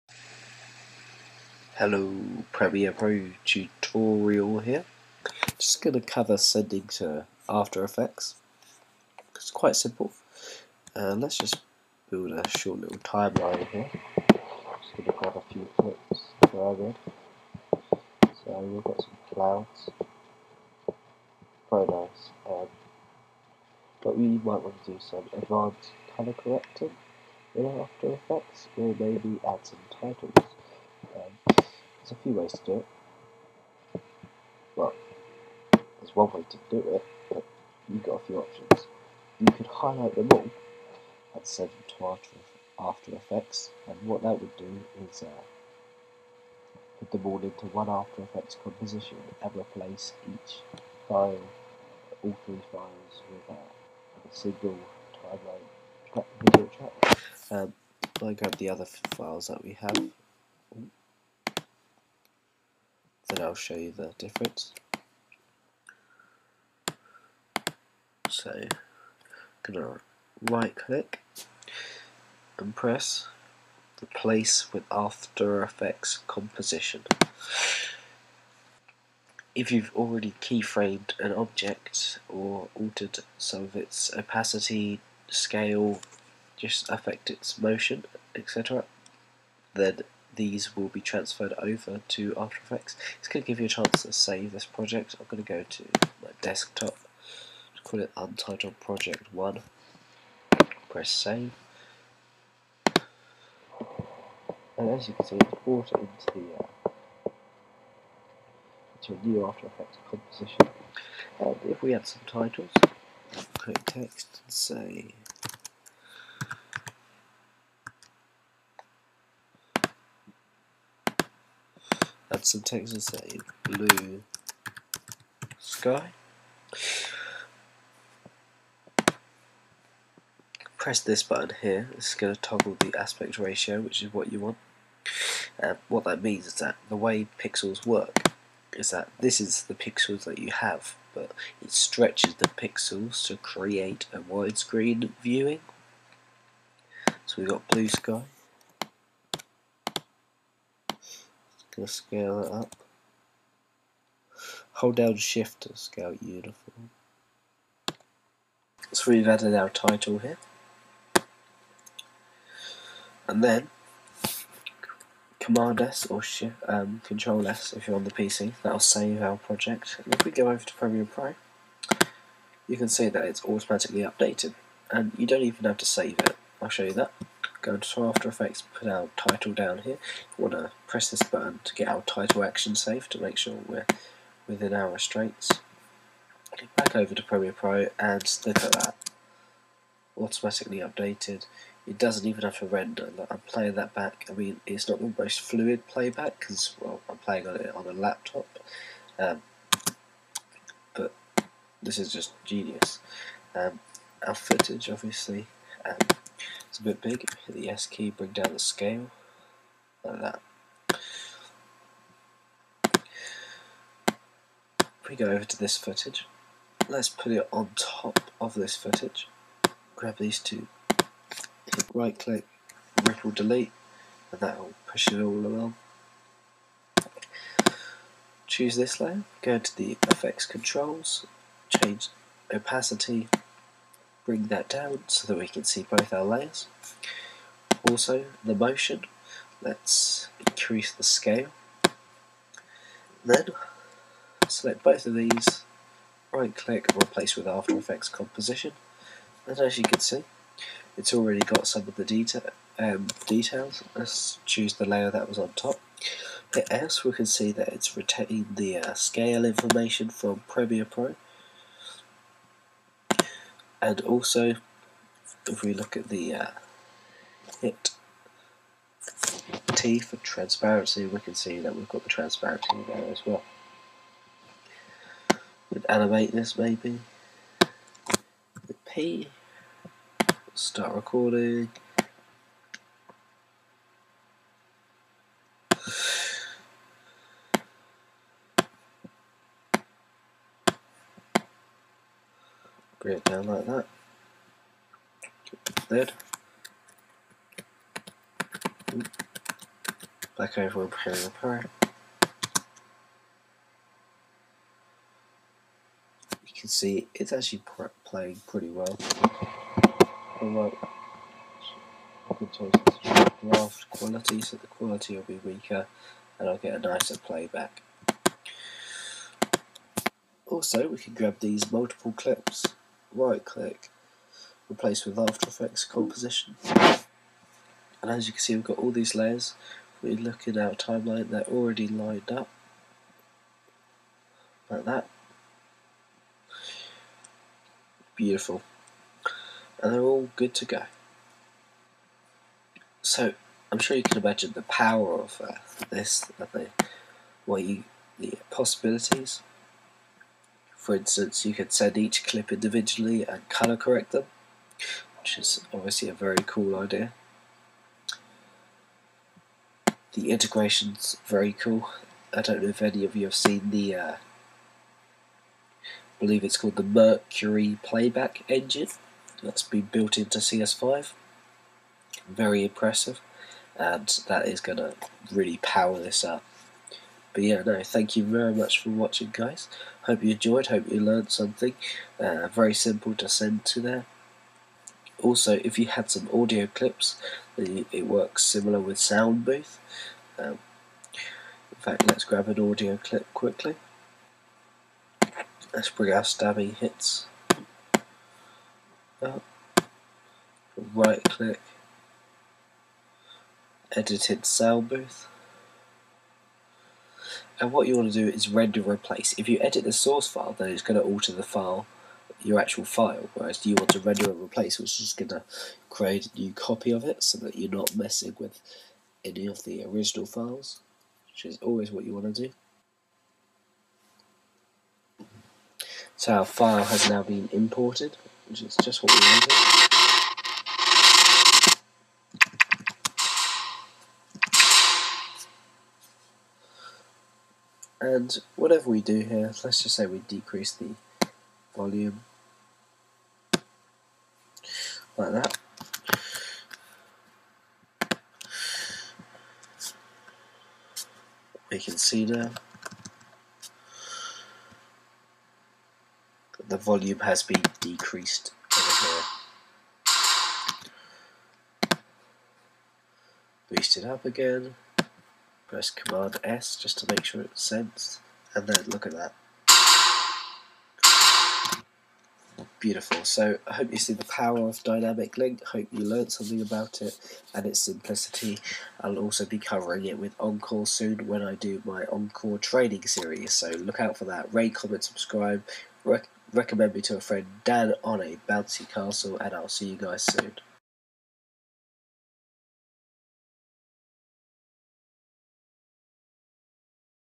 Premiere Pro Basics Tutorial : Using Dynamic Link
( HD Voice Tutorial )
Adobe Premiere Pro CS4 Tutorial
In this video we will cover:
-Sending footage / clips to After Effects
-Properties of Dynamic Link
-Sending audio to Soundbooth